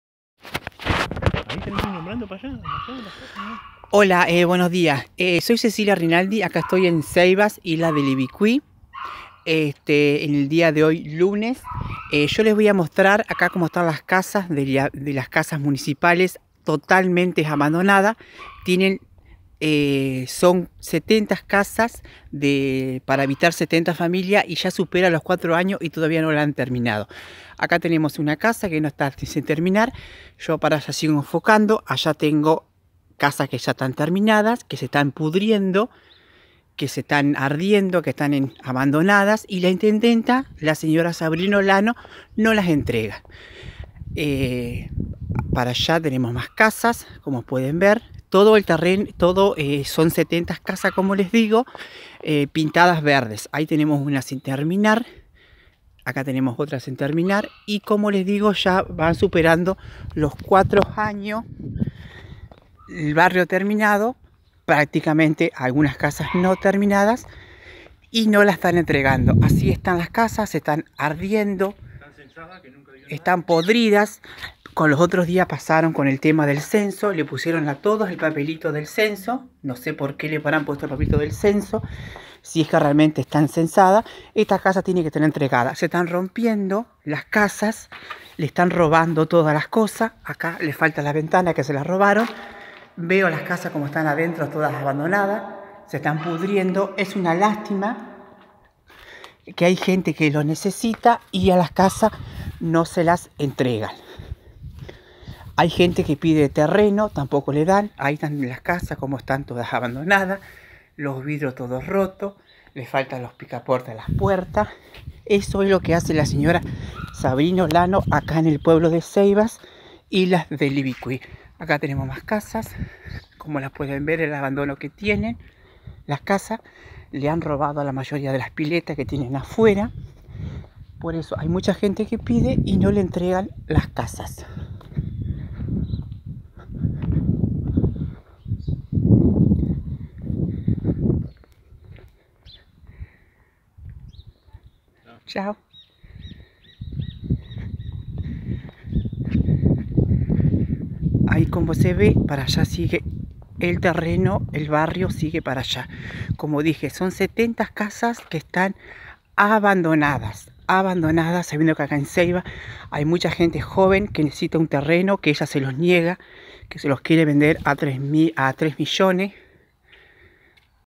Cecilia Rinaldi: “La Intendenta de Ceibas vende a 3 millones de pesos cada terreno”.
Cecilia Rinaldi es nacida y criada en la localidad de Ceibas provincia de Entre Ríos, y contó a DIARIO NOVA, que la Intendenta Sabrina Daniela Olano la recibió ayer 12 de julio en su despacho alrededor de las 10:00 de la mañana,-Rinaldi expresó, le fui a plantear sobre las (70) casas que se encuentran en la localidad sin entregar a las familias, la Señora Olano me argumentó que aún no se han terminado de construir por ese motivo no se han entregado.
En este mismo contexto aseguró Rinaldi que la Intendenta le dijo que tiene disponible terrenos para la venta a unos 3 millones de pesos, el que lo quiera comprar, y lo necesite "lo va a comprar" manifestó Sabrina Daniela Olano a la vecina.